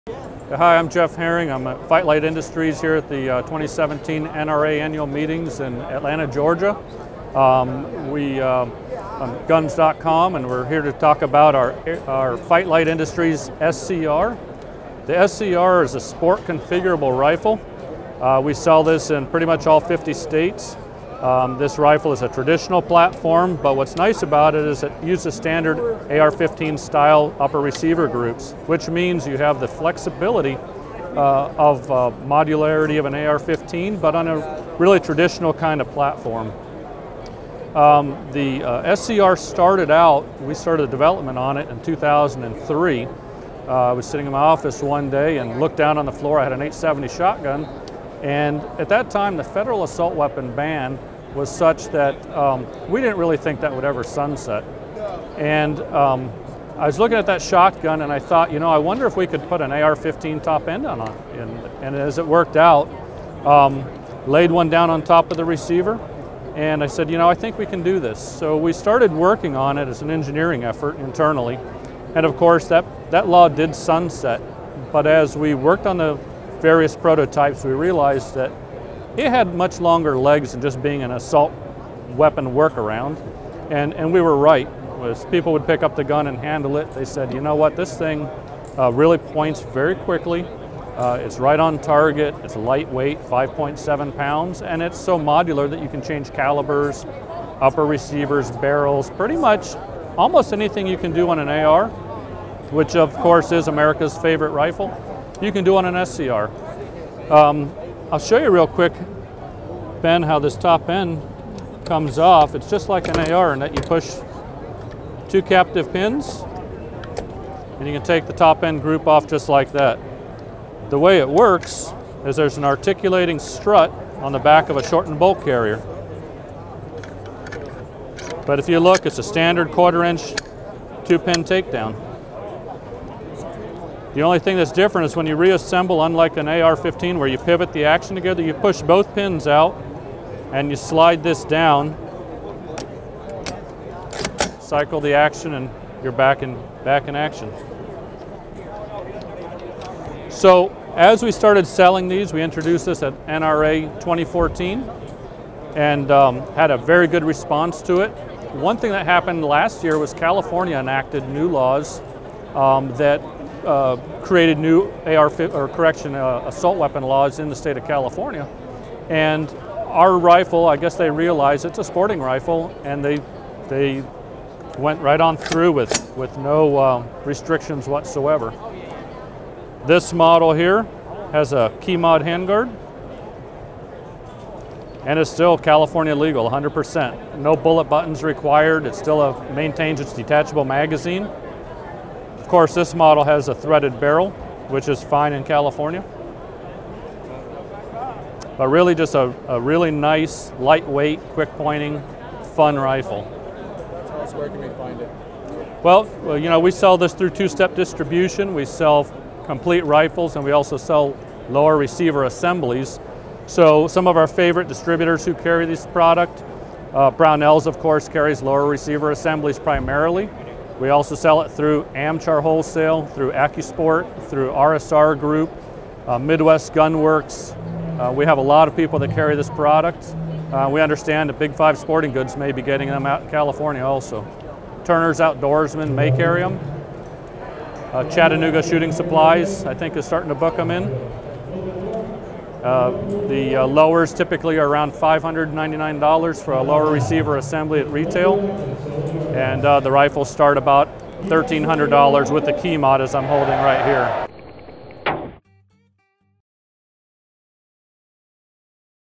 FightLite SCR rifles at NRAAM 2017
Melbourne, Florida-based FightLite, through its SCR series rifles, has long been giving those whose local laws hinder traditional AR platform ownership a seat at the table

Geoffrey A. Herring originally started the company as ARES Defense Systems in 1997 and last year changed the name to FightLite to better reflect their products. Guns.com met with Herring over the weekend at the 146th National Rifle Association Annual Meetings and Exhibits at the Georgia World Congress Center in Atlanta and spoke with him about the SCR line of “featureless” rifles that use a standard AR upper and modded lower with a sporting (no pistol grip/collapsible butt) Monte Carlo-profile stock.

The guns, originally designed to be compliant with the now-sunsetted federal assault weapon ban, coincidentally are still compliant with New York’s 2013 SAFE Act and have become a big hit (as have other featureless designs) in post-Gunmageddon California.

With five-round magazines standard — they also accept any regular AR mag — the receivers are made from 7075-T6 forgings and use mil-spec Carpenter 158 bolts and 4140 chro-moly 1:9 twist barrels. Stock and forearms come in Magpul MOE with M-LOK systems as well as KeyMod with MSRP running from $1132 upward.

They also sell lower receiver assemblies through a variety of dealers that, if you shop around, can be found for under $500.

Coincidentally, on the other end of the spectrum, FightLite also makes the MCR belt-fed AR.